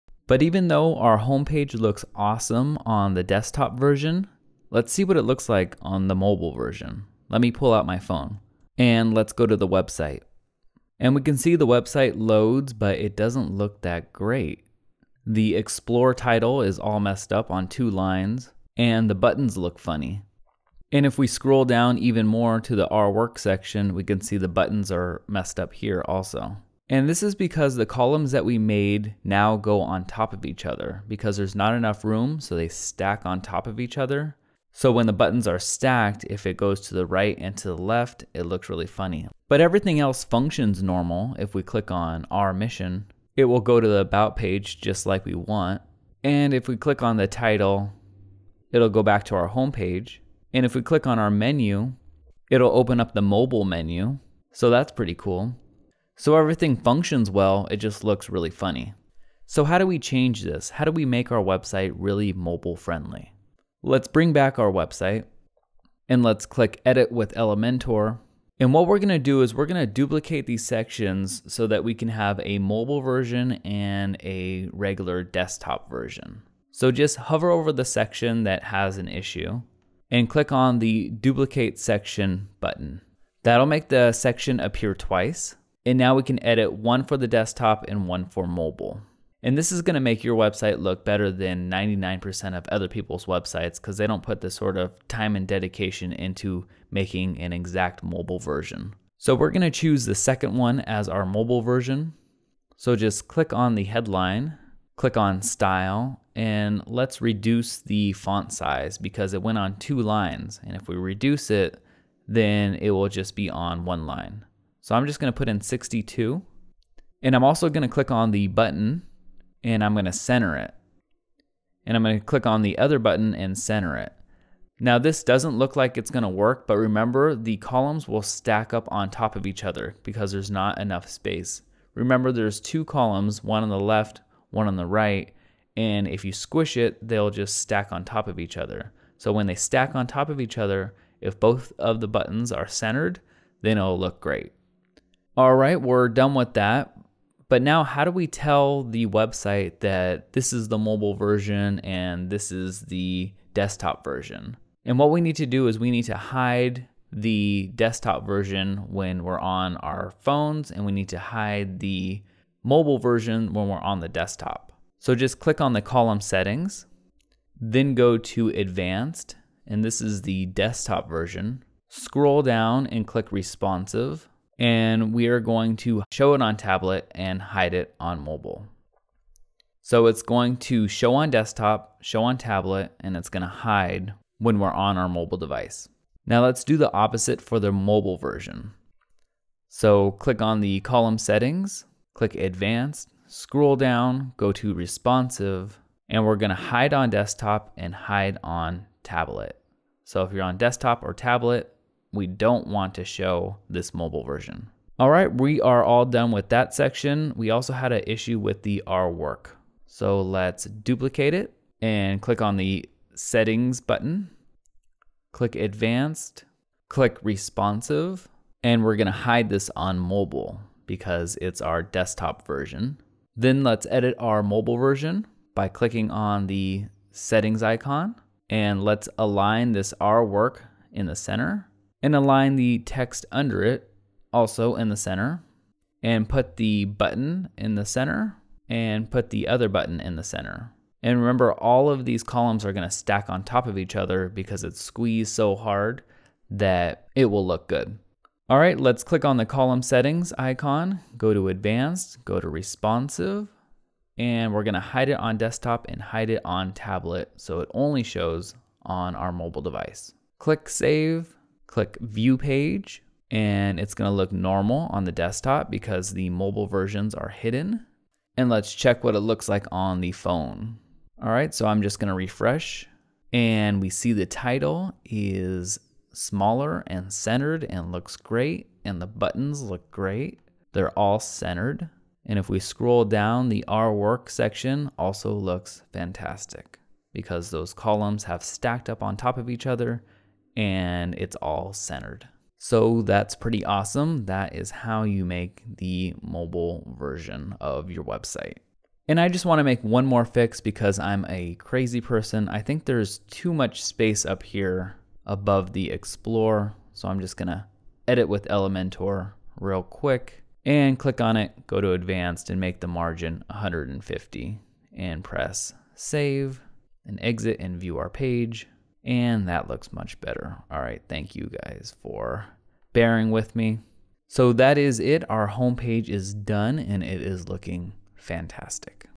Responsive Website In WordPress #20
Make your website mobile friendly and responsive in WordPress.  We are using the elementor plugin with OceanWP theme.  The best combo in the world so far.  This will work on desktops, phones, tablets etc.